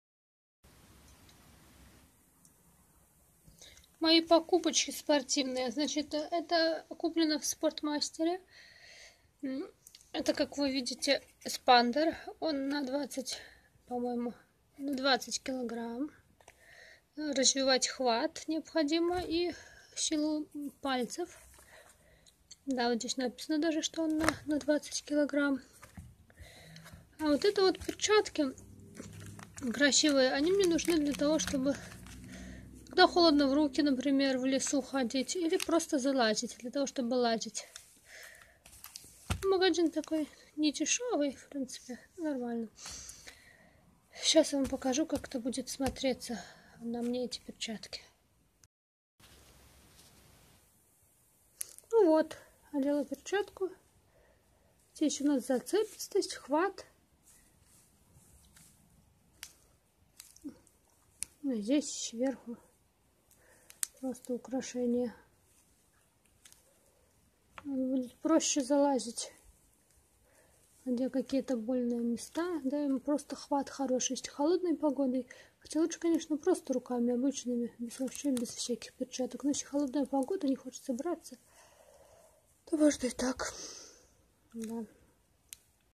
спортмастер перчатки эспандер
В магазине  спортмастер купила беспалые перчатки, эспандер. Это все для лазания по деревьям мне надо. Тренирвовать хват.
Quetzal